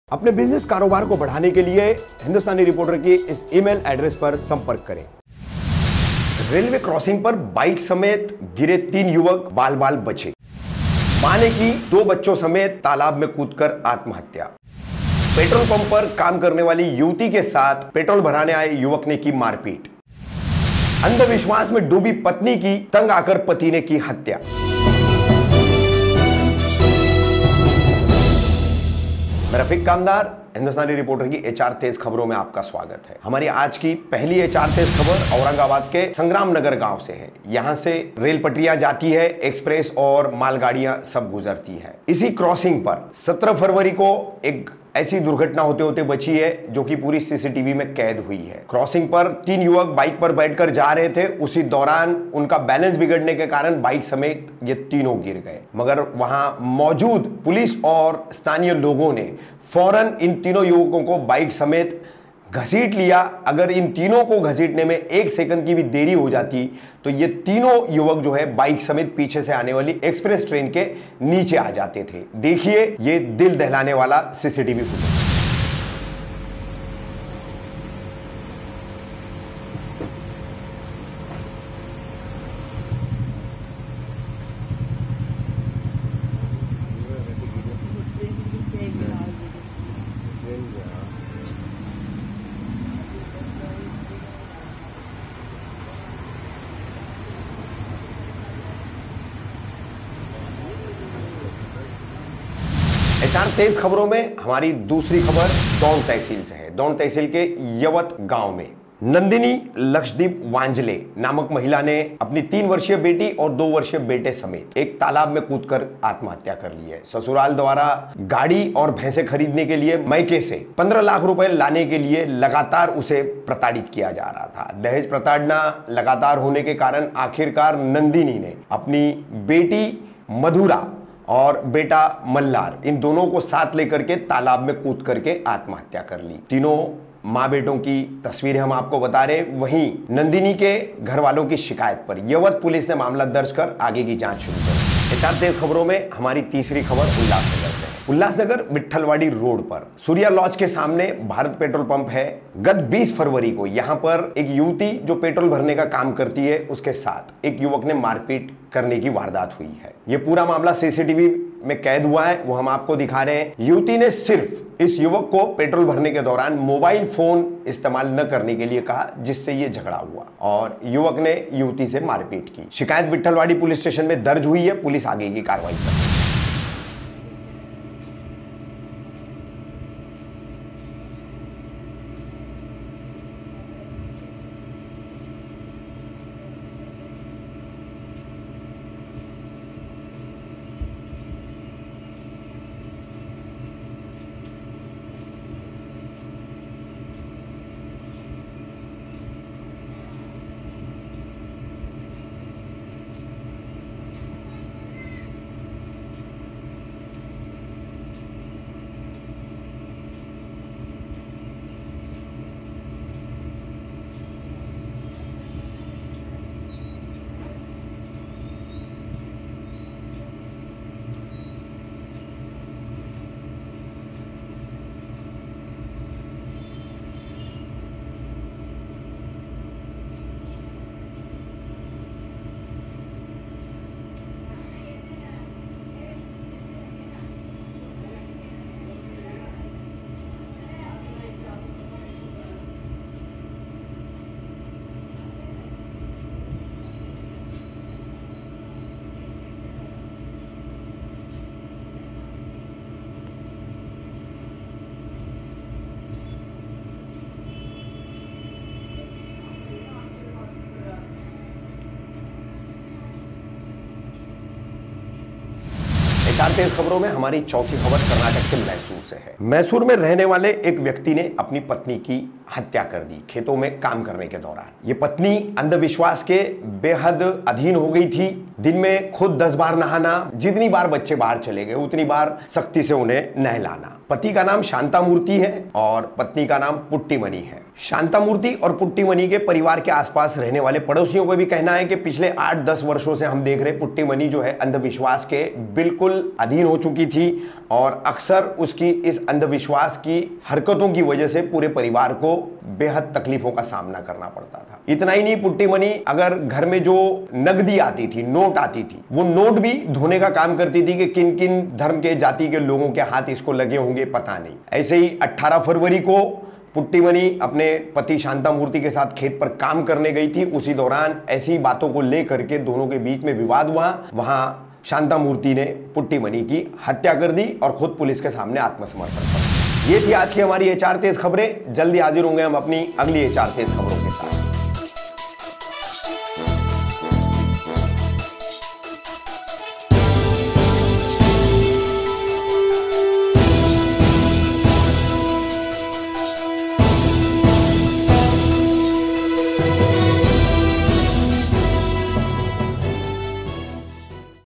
एचआर तेज खबरें | 146 | रेल्वे क्रॉसिंग पर बाईकसमेत गिरे तीन युवक बाल बाल बचे
एचआर तेज खबरें

रेल्वे क्रॉसिंग पर बाईकसमेत गिरे तीन युवक बाल बाल बचे

दहेज प्रताड़ना के कारण मां ने की दो बच्चों समेत आत्महत्या

पेट्रोल पंप पर काम करनेवाली युवती से युवक ने की मारपीट

अंधविश्वास में डूबी पत्नी की पति ने की हत्या

Mic We Use